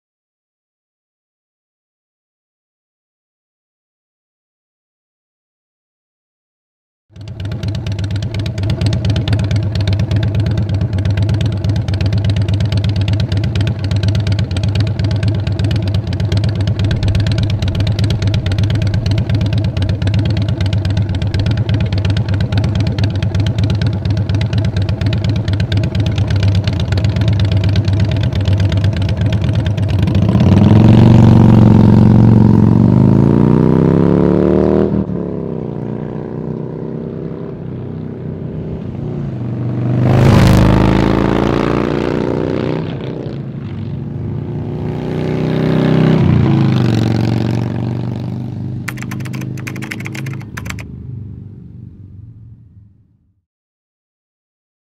Vance & Hines Black Big Shots Staggered Exhaust | Available at J&P Cycles
Vance & Hines Black Big Radius 2-into-1 Exhaust System
• Traditionally viewed as the kind of pipe merely designed for performance, Big Radius 2-into-1 breaks the mold, delivering white-knuckle performance with the style that has won over the motorcycle public
• Incorporating blue-proof technology across the entire length of the pipe, Big Radius 2-into-1 will keep its showroom finish whether at the drag strip or on the boulevard
• The flat plane mounting panel, for the 12mm oxygen sensors, positions sensor deeper into exhaust stream, providing enhanced data feedback to the ECM
• Includes mounts for factory O2 sensors as required
• Will requires removal of passenger footpeg, if applicable, in order to mount exhaust.